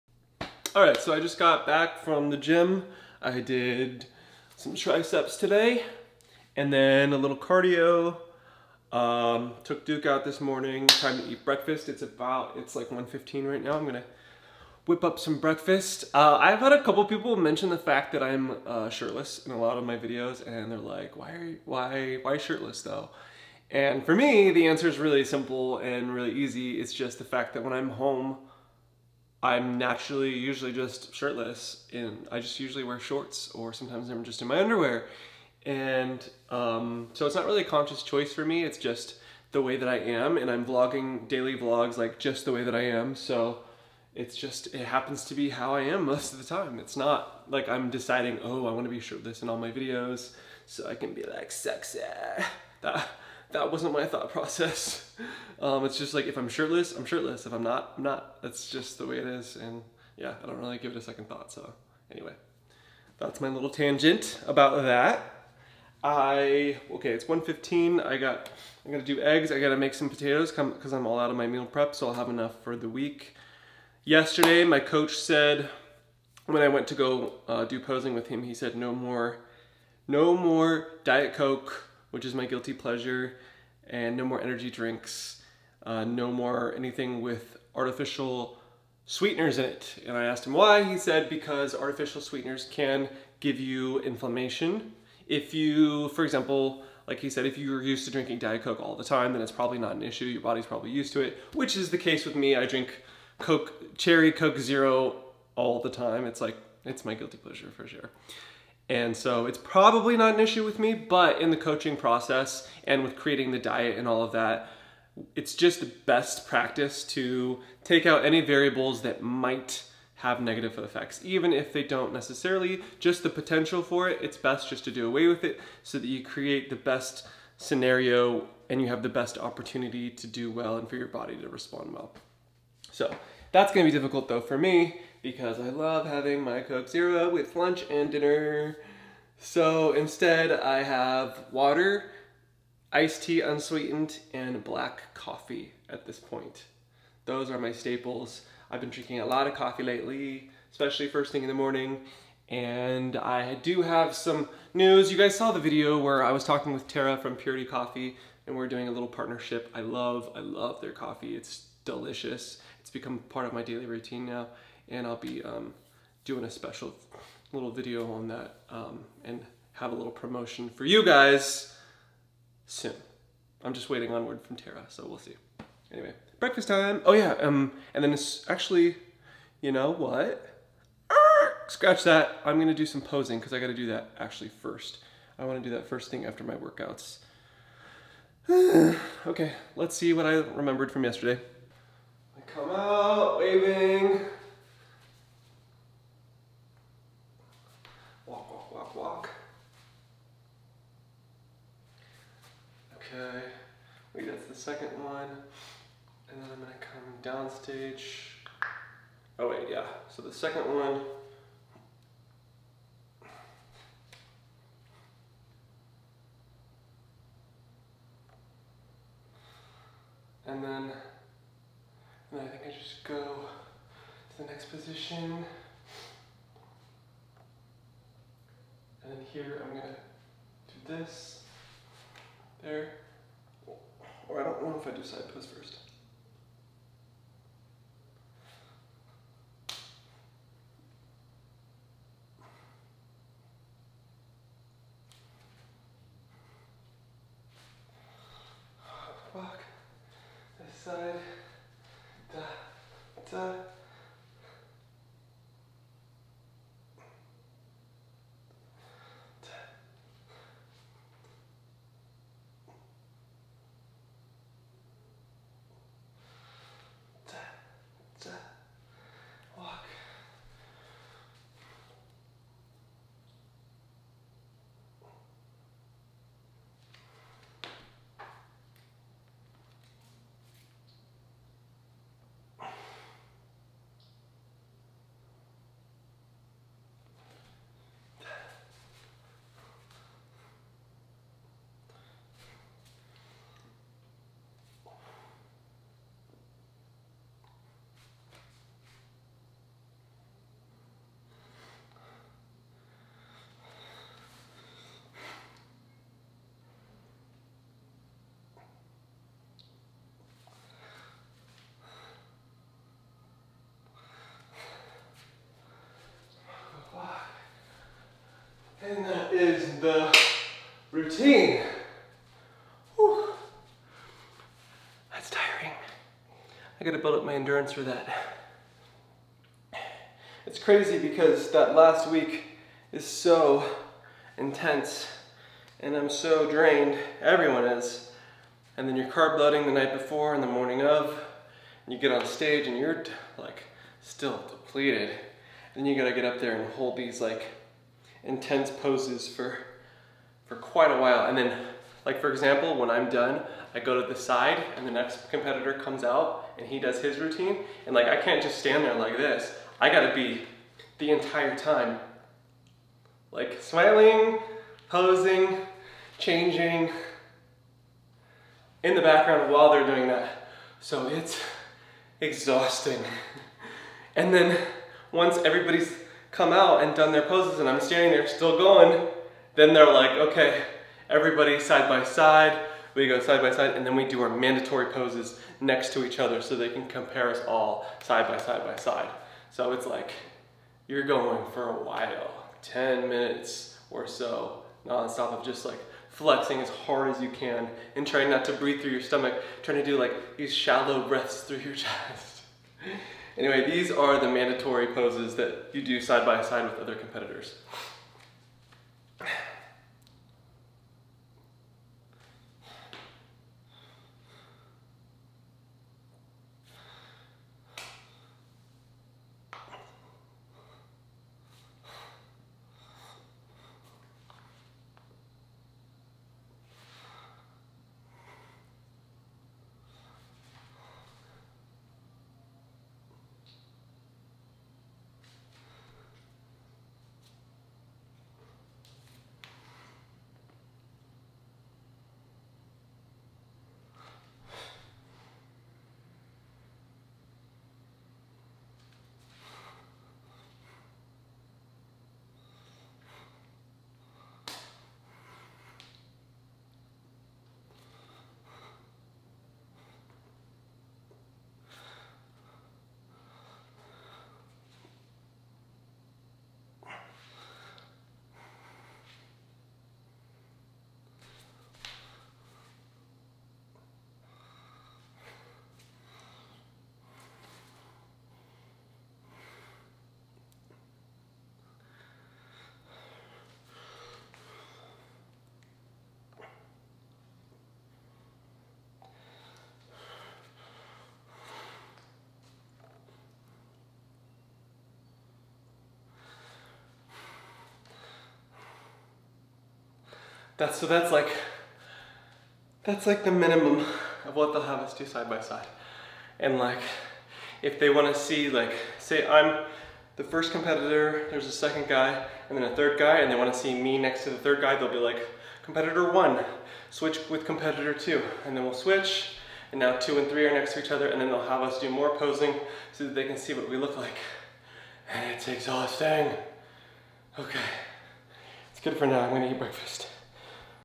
Practicing Posing at Home - Daily Vlog 10-01
Day 9 of 20 days until my men's physique bodybuilding competition. I'm going to daily vlog as a way of showing you guys the final push and to keep myself accountable. And you can get a sneak peak of what my daily routines are like ;)